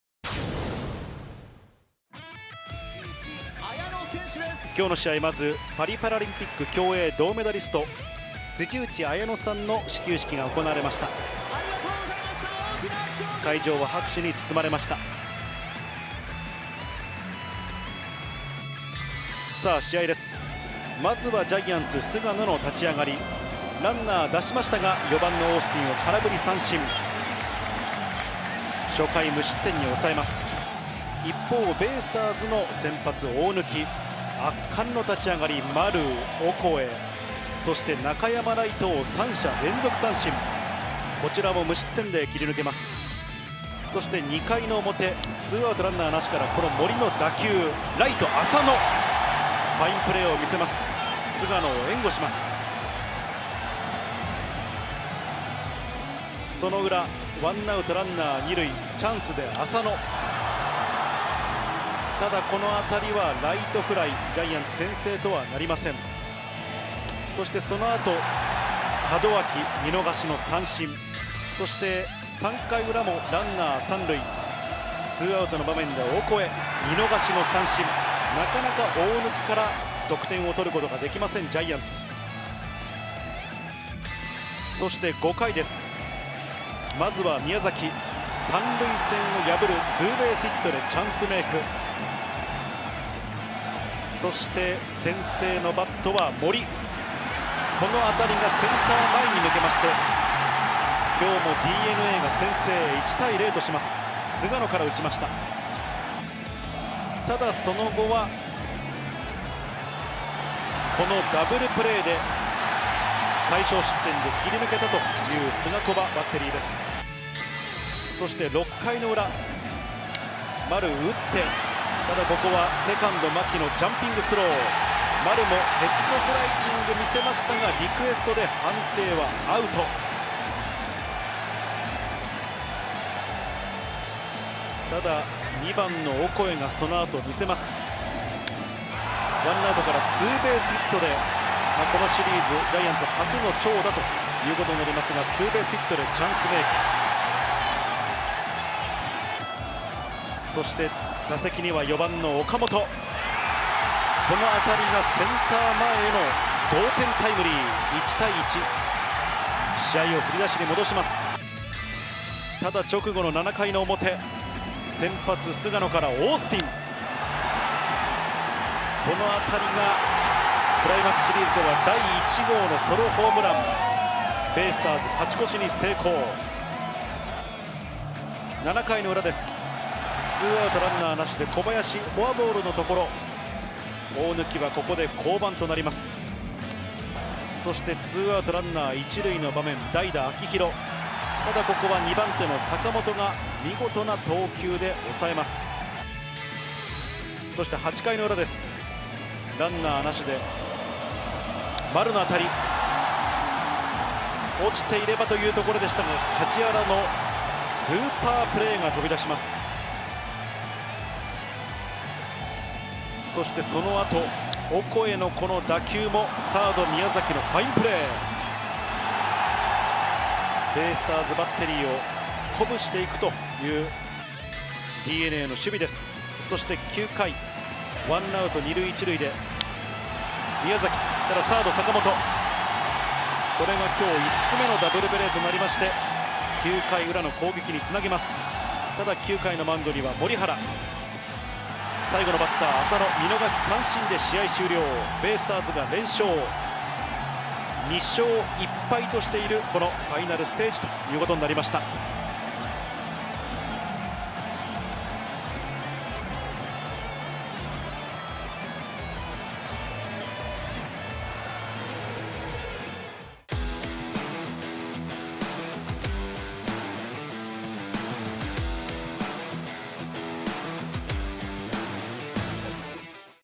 【ハイライト・10/17】DeNA CS4連勝！大貫7回途中1失点の好投＆オースティンＶ弾【巨人×DeNA】【CSファイナル第2戦】
2024年10月17日(木)
CSファイナルステージ第2戦